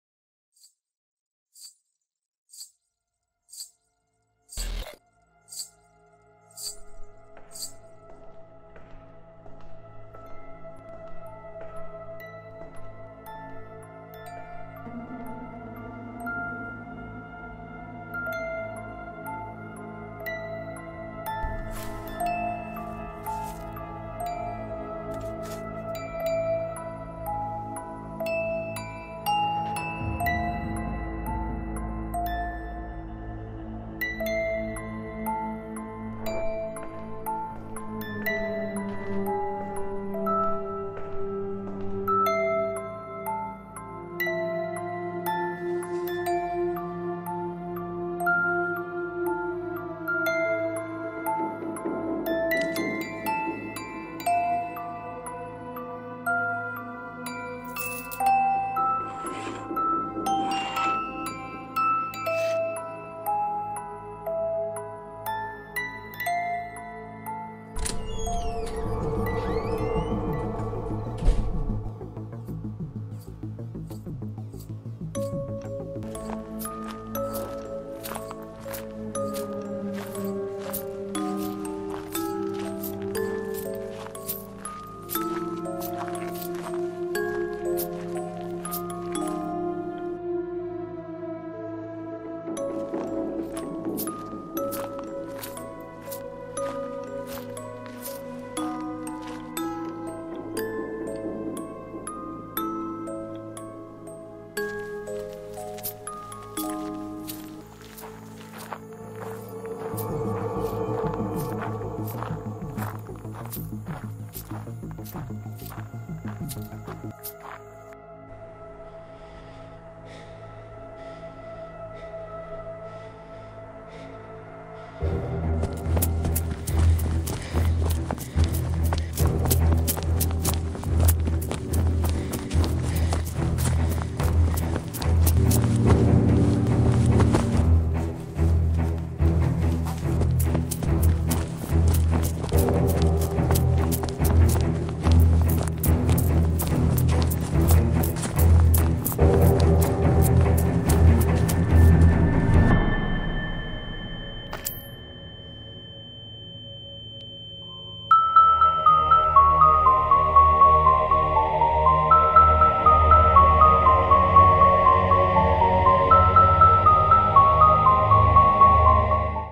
The girl in the Road |Short Film| Perception | IIT Mandi
The girl in the road, a short horror-suspense film by Perception the videography club of IIT Mandi. A film which you can perceive as you want. Let us know your views and opinions in the comments section.